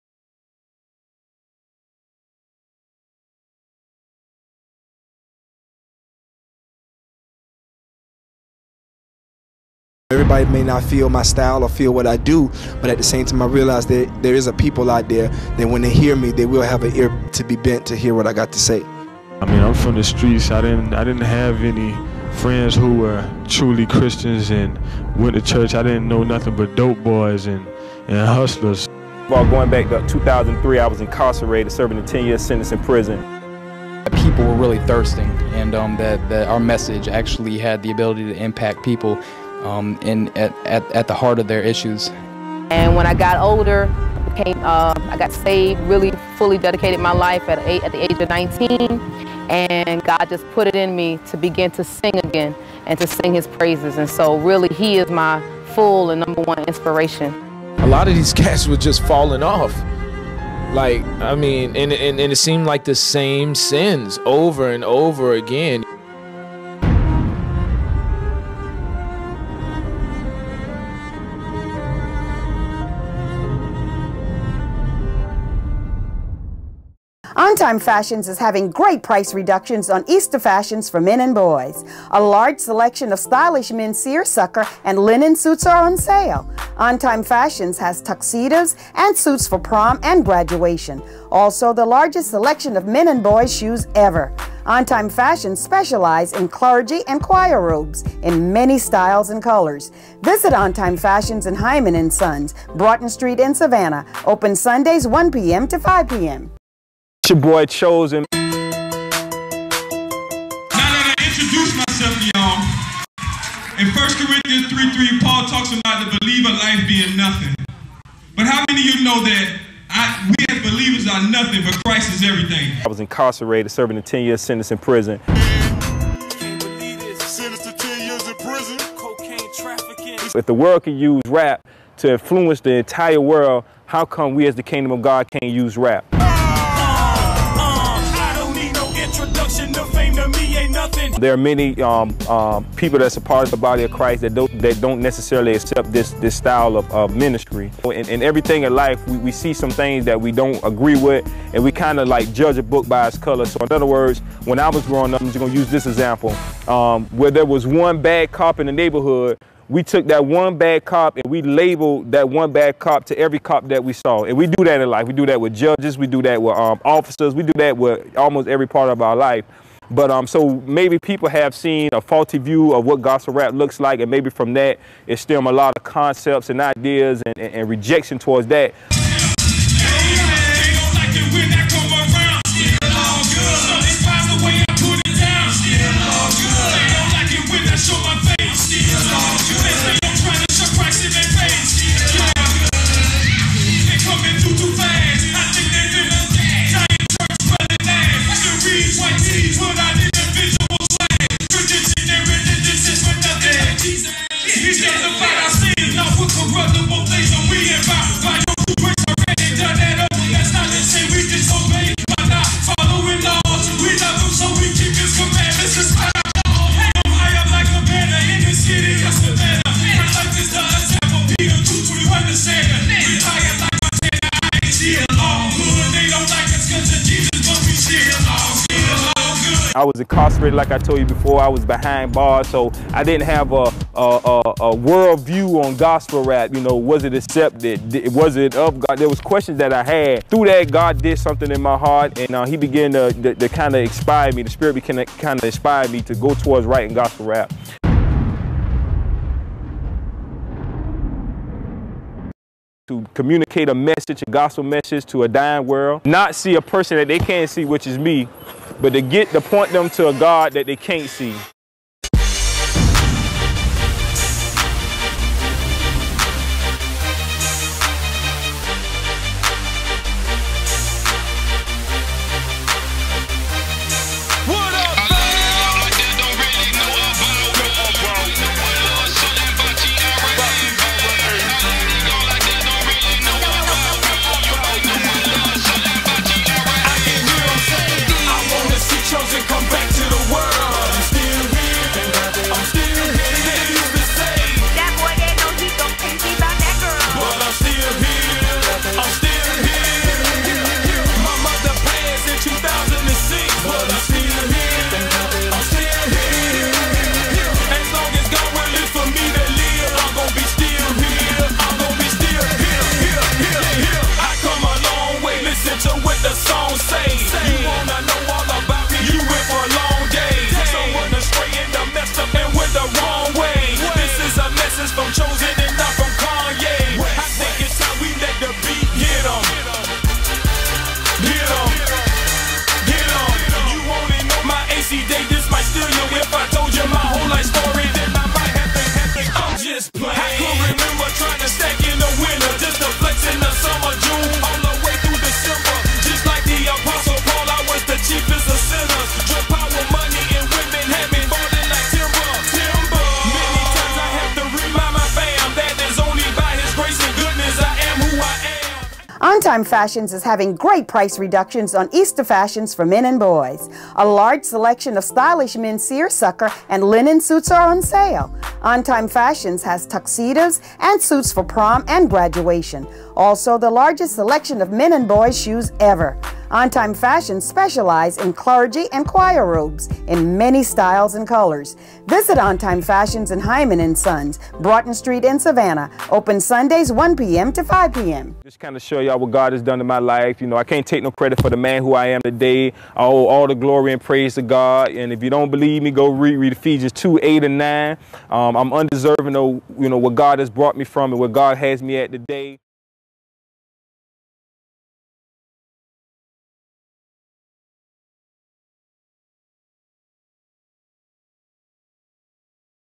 Chosen-Based On A True Story
Meet Chosen a member of CCM. A christian rap music based ministry.